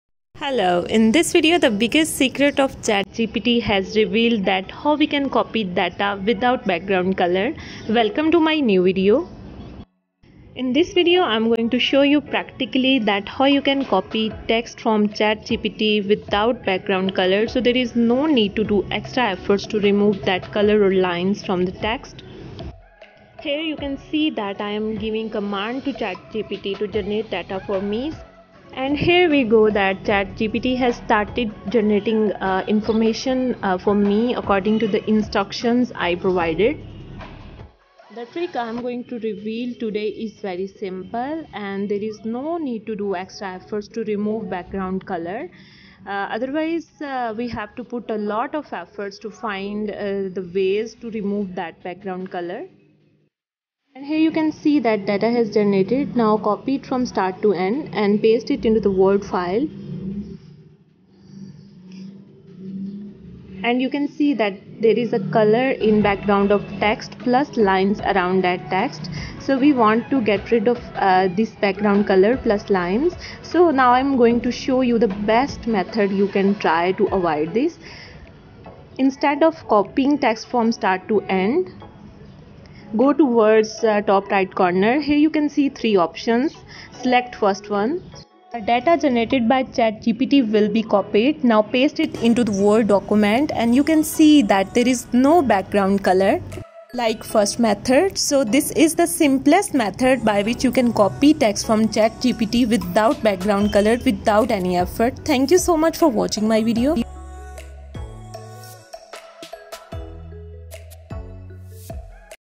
How to Copy Text from ChatGPT to Microsoft Word without Background Color removal effort
Remove_background_color
Writing_ tools
AI-powered_text_generation
Optimizing_ChatGPT_Output:
How to Remove Background Color in Microsoft Word"

Discover a simple yet effective hack to optimize your ChatGPT experience! Are you tired of dealing with the dark background color that appears when you copy text from ChatGPT into Microsoft Word? Look no further! In this video, I'll show you how to effortlessly transfer text generated by ChatGPT to Microsoft Word without any background color. By using the copy option on the upper right corner of ChatGPT, you'll achieve a clean and professional look for your text in Word. Don't miss out on this SEO-friendly video packed with valuable tips and tricks to boost your productivity.